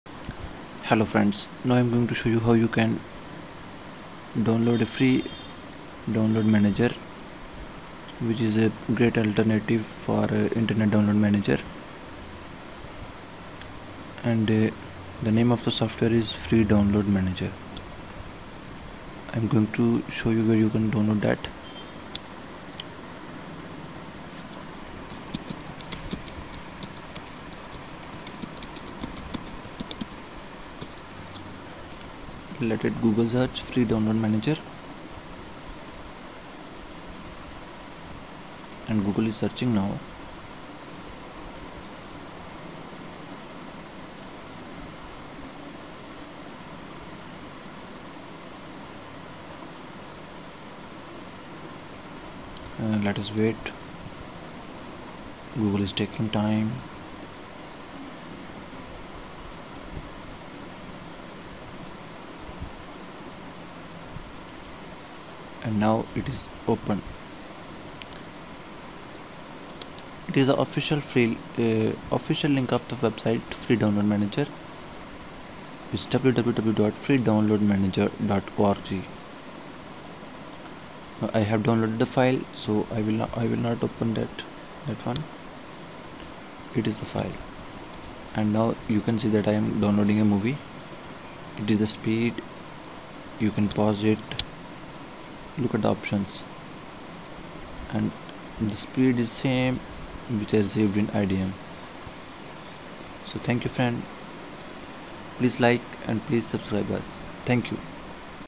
Best alternative for IDM internet Download manager
Hello dear friends. are you tired from the crack keys for internet download managers? if yes then you are at the right place.